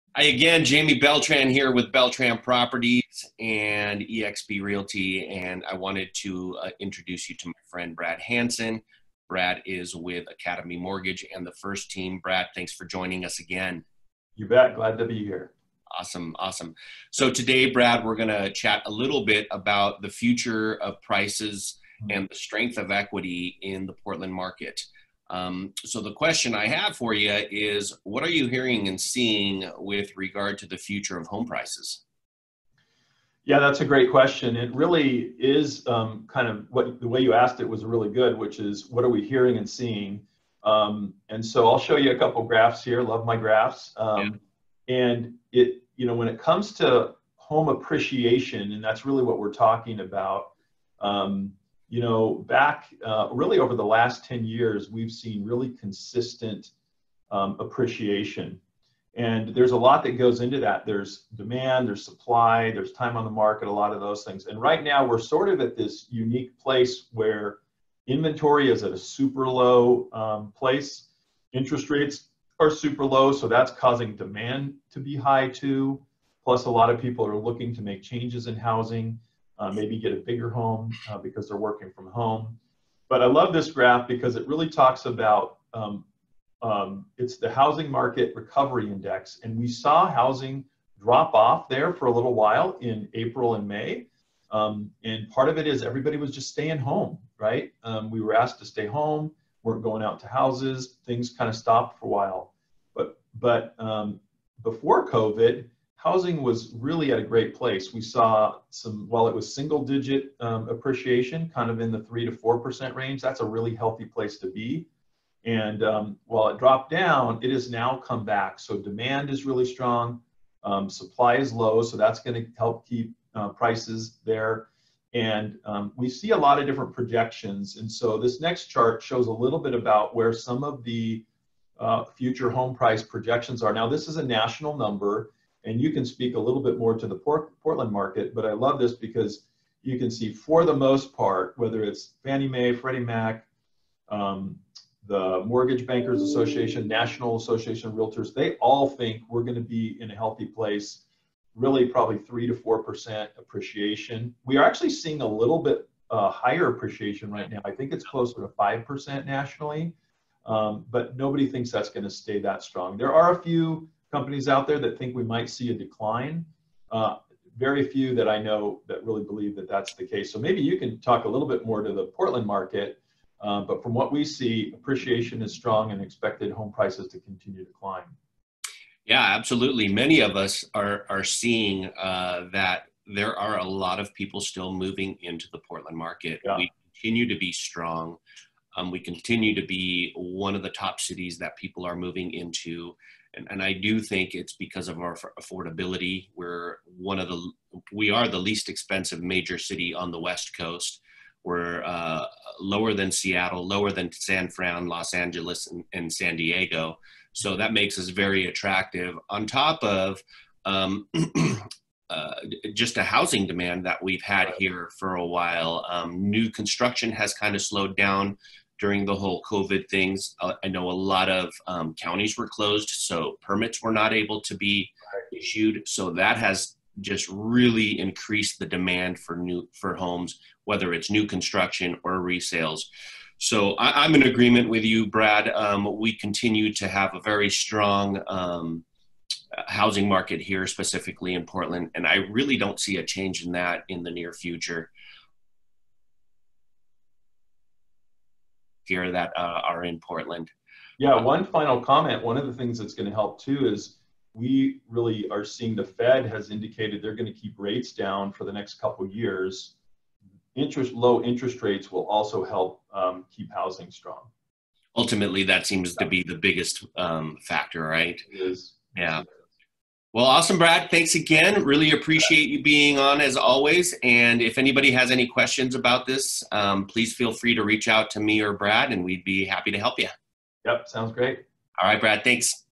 Jaimy Beltran and Brad Hansen Talk About The Future of Real Estate Prices
Brad Hansen of Academy Mortgage shows how the housing market has recovered since March and April of 2020 and what is expected down the road.

We aim to make you a client for life. Call us and see for yourself. We look forward to serving your every real estate need.